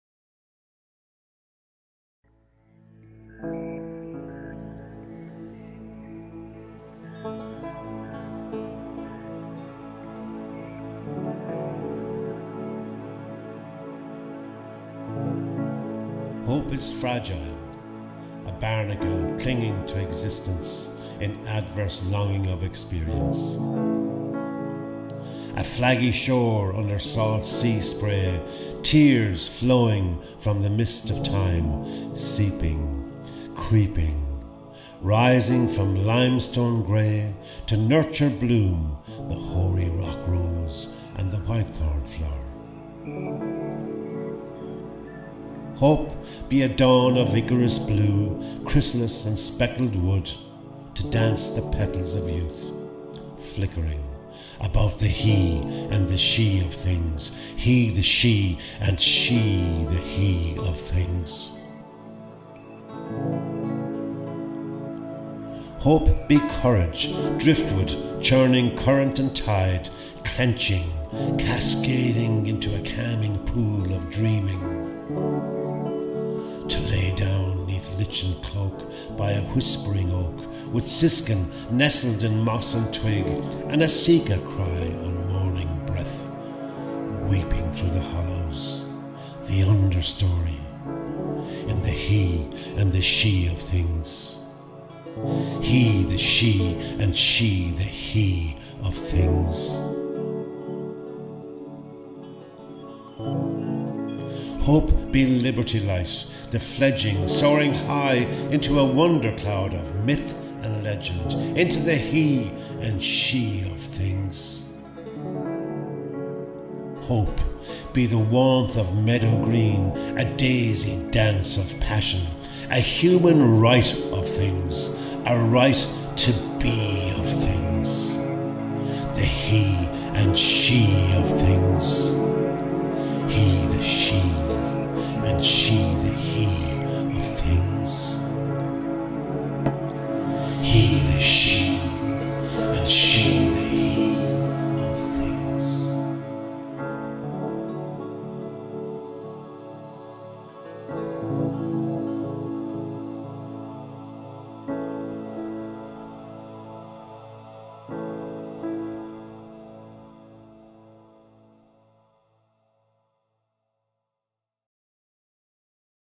Hope A poem - Mike Hanrahan - Co Clare 2023
HOPE – Mike Hanrahan -Trinity College, Dublin-May 2023
“Where creativity and science come together, lies a bridge called hope.”
This poem is inspired by, and dedicated to, my GBHI cohort at Trinity College, Dublin and UCSF, San Francisco.
Hope – Dóchas – Esperanza Presented as an abstract at
GBHI Annual Conference May 2023: Mexico City.
Written and created by Mike Hanrahan

HOPE
Hope is fragile
a barnacle clinging to existence
in adverse longing of experience
a flaggy shore under salt sea spray
tears flowing from the mist of time
seeping, creeping
rising from limestone grey
to nurture bloom
the hoary rock rose and whitethorn flower

Hope be a dawn of Icarus blue,
Chrysalis and Speckled wood,
To dance the petals of youth
flickering
about the he and she of things
he the she and she the he of things

Hope be courage
driftwood churning current and tide
clenching, cascading
into a calming pool
of dreaming
to lay down neath lichen cloak
by a whispering oak
with siskin nestled in moss and twig
and a sika cry on morning breath
weeping through the hollows,
 the understory
in the he and she of things

Hope be liberty light
The fledging, soaring high
Into a wonder cloud
Of myth and legend
Into the he and she of things

Hope be the warmth of meadow green
A daisy dance of passion
A human right of things
A right to be of things
The he and she of things
He the she she the he of things